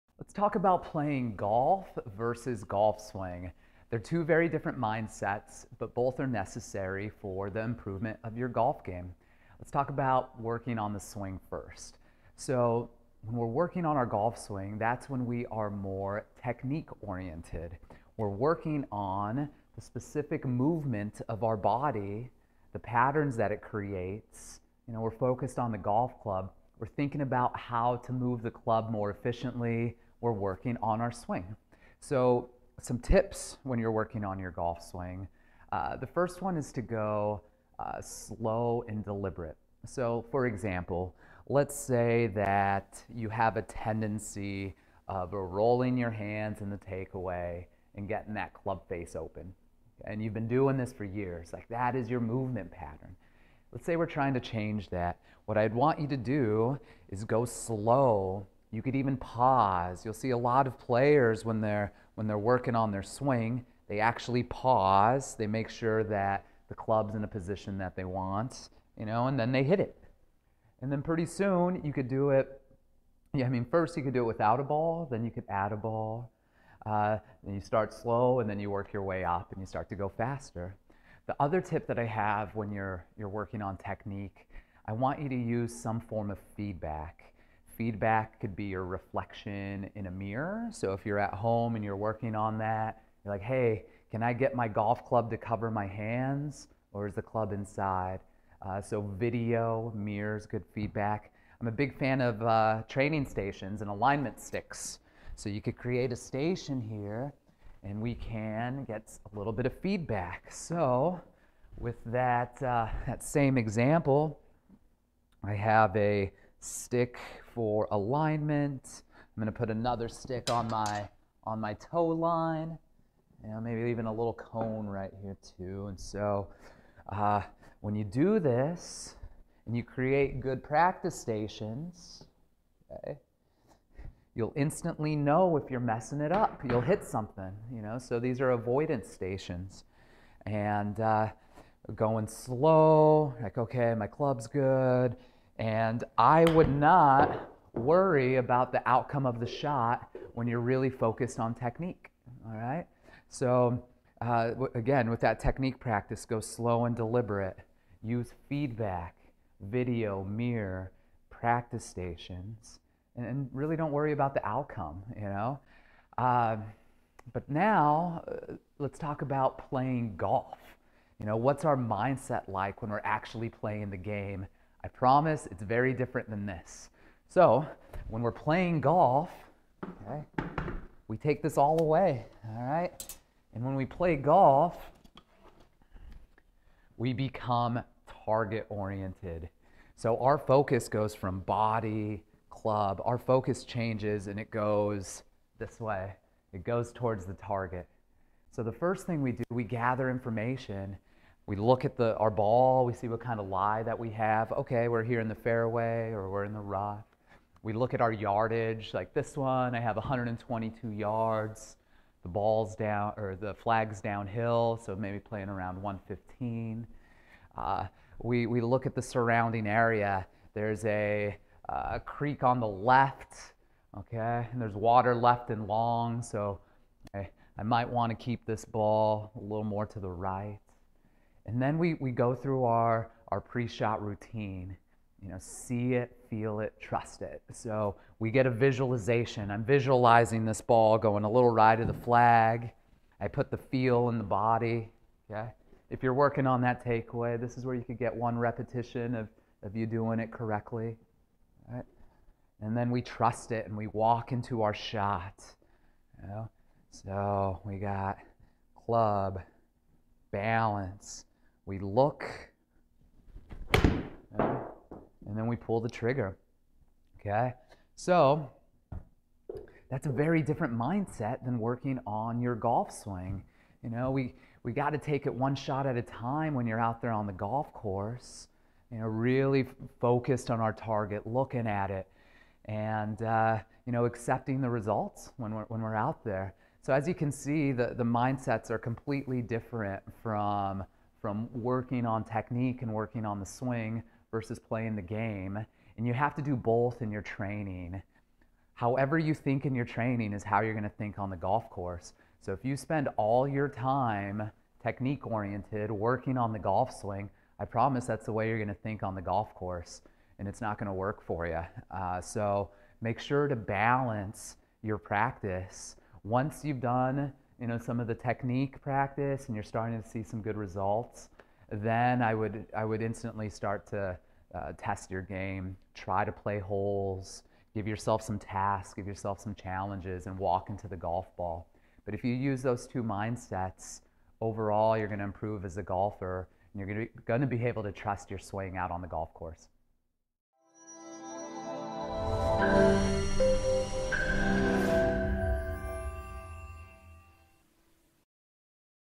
Playing Golf Vs. Golf Swing
Learn the different mindsets necessary to improve your golf swing vs. playing the actual game of golf.